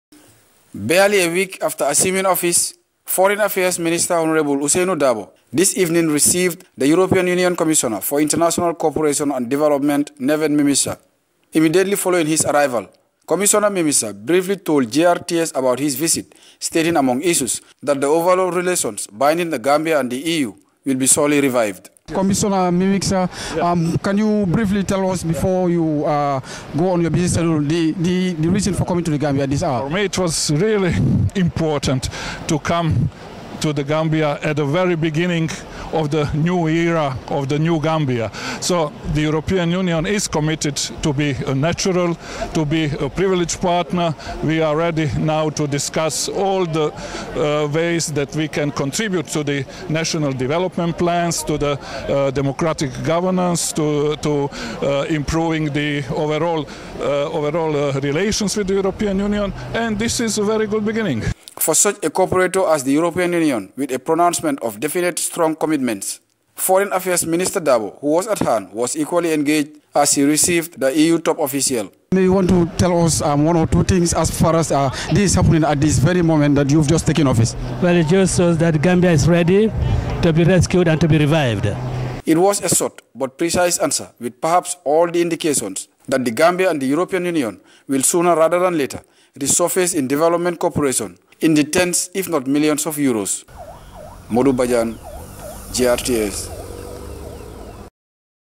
Modou Bajan: EU Commissioner Neven Mimica's Visits The Gambia
EU Commissioner Neven Mimica's Visits The Gambia Ahead of More Foreseen Cooperation Between The Two Parties